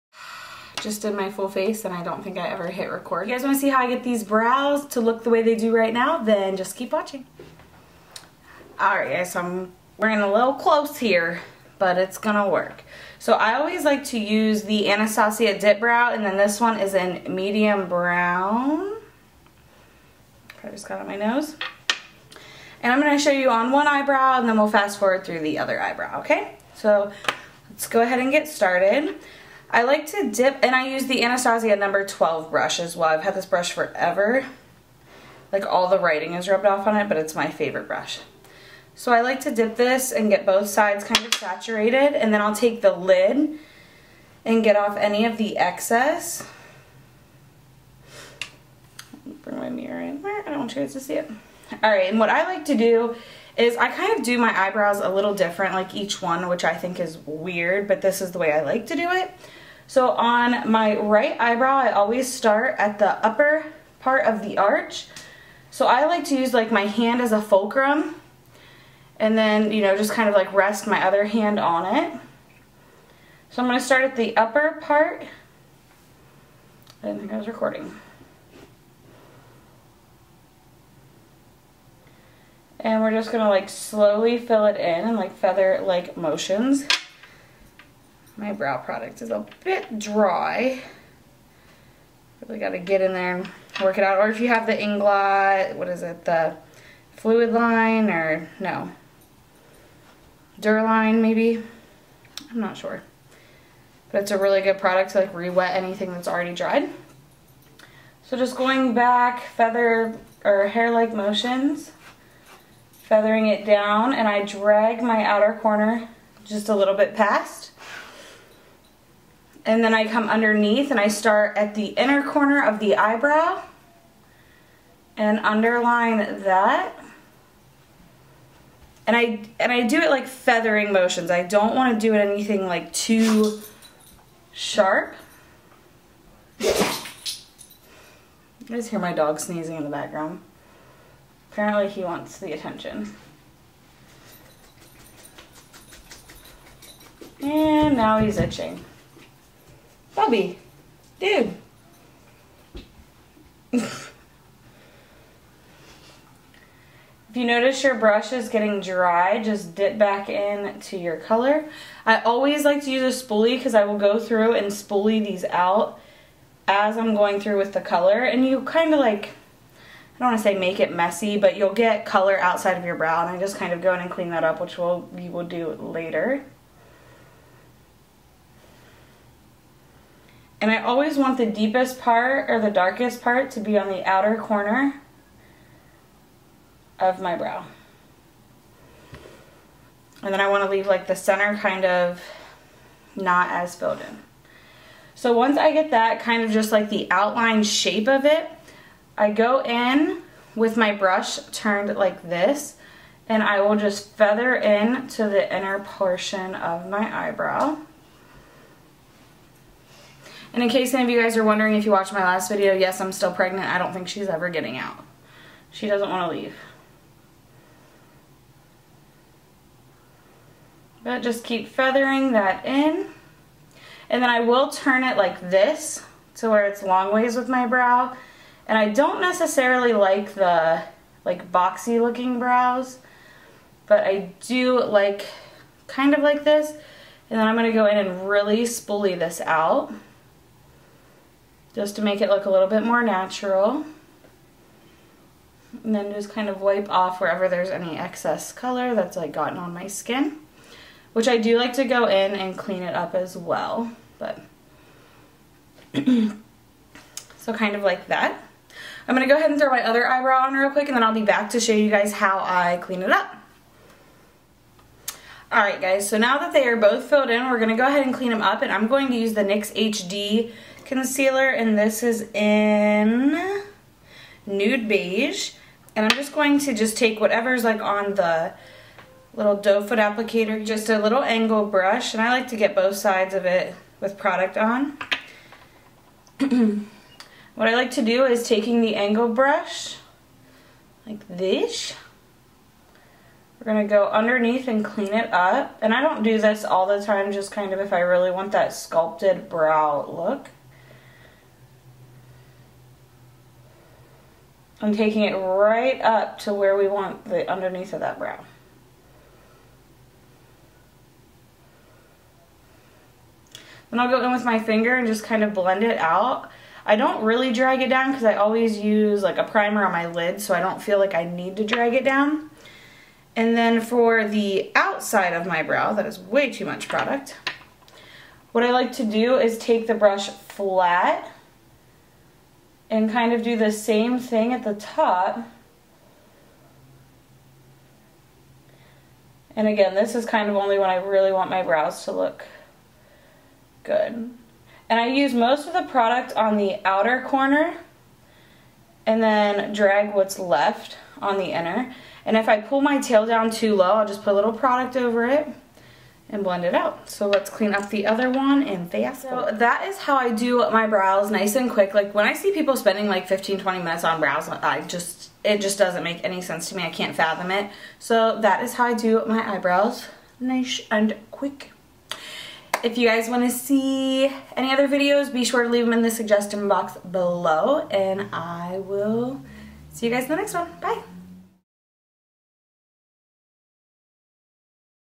QUICK, Perfect, Easy Eyebrows | CourtneyMUA32
Are you sick and tired of watching people take 20 minutes on their eyebrows? I know I am! It is not necessary AT ALL! I hope this video helps you cut down on your eyebrow time!